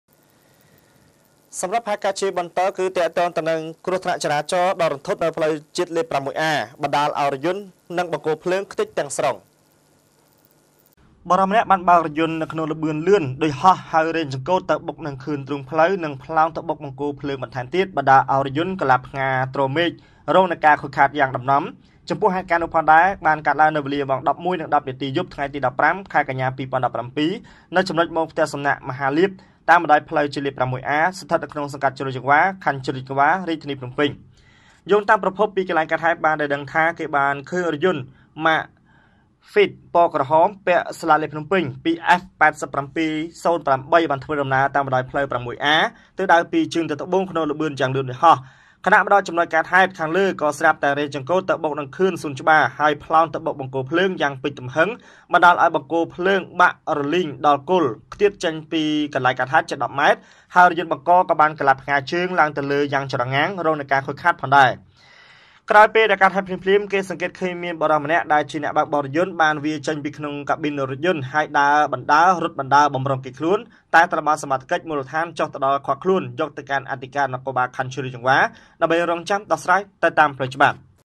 គ្រោះថ្នាក់ចរាចរណ៍ ដ៏រន្ធត់នៅផ្លូវជាតិ លេខ6A បណ្ដាលឲ្យរថយន្ត
គ្រោះថ្នាក់ចរាចរណ៍ ដ៏រន្ធត់នៅផ្លូវជាតិ លេខ6A បណ្ដាលឲ្យរថយន្ត និងបង្គោលភ្លើង ខ្ទិចទាំងស្រ៊ុង